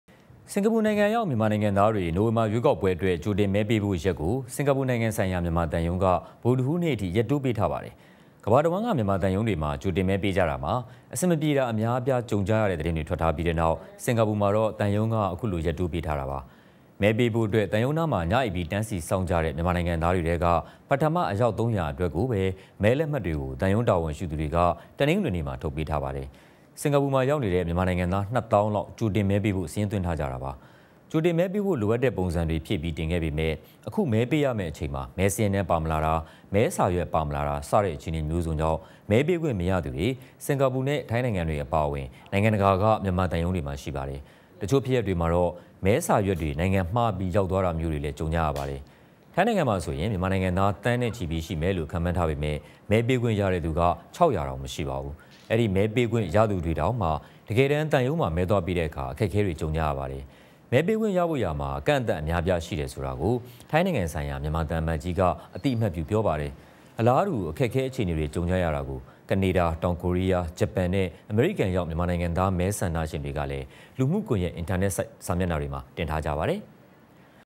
တနလၤာေန႔ ျမန္မာတီဗီြသတင္းမ်ား
စကၤာပူႏိုင္ငံေရာက္ ျမန္မာႏုိင္္ငံသားေတြ ႏို၀င္ဘာေရြးေကာက္ပြဲအတြက္ ႀကိဳတင္မဲေပးဖို႔ရက္ကို ျမန္မာသံ႐ံုး က ဗုဒၶဟူးေန႔အထိ ရက္တိုးေပးထားပါတယ္။ သတင္းဌာနေတြအေနနဲ႔ မီဒီယာက်င့္၀တ္ေတြနဲ႔အညီ ေရးသား ထုတ္လႊင့္သြားၾကဖို႔ ျပည္ေထာင္စုေရြးေကာက္ပြဲေကာ္မရွင္က ေအာက္တိုဘာလ ၁၈ ရက္ေန႔စြဲနဲ႔ သတင္း ထုတ္ျပန္ထားပါတယ္။